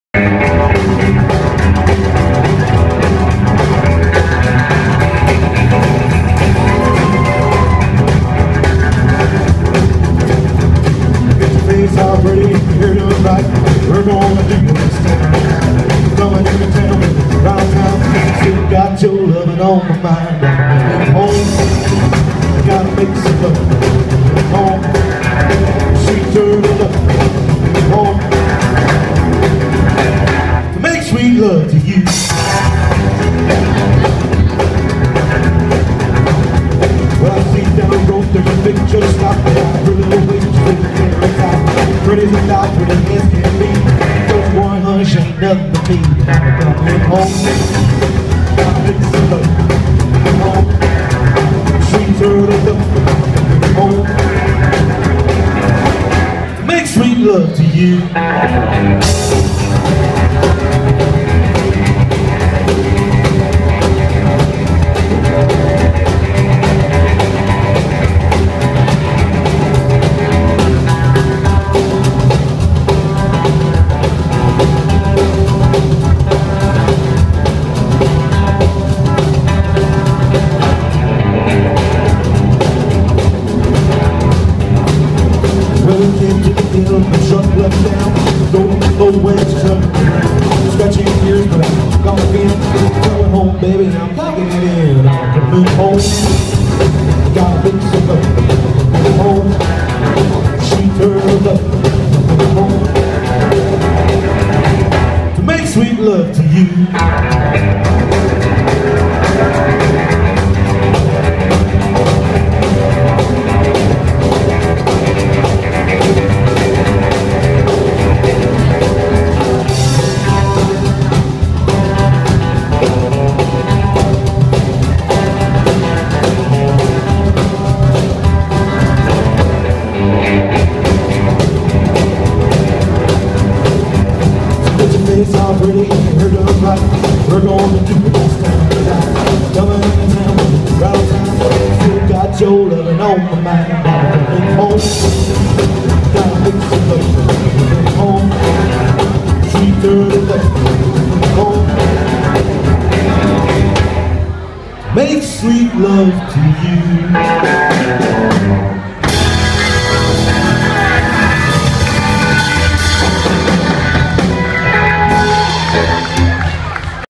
Doggone Honkabillies@Witham Public Hall, New Year's Eve 2015 (8)
Witham Public Hall dance featuring Doggone Honkabillies with guest Sam French on upright bass, performing for the new year's celebrations.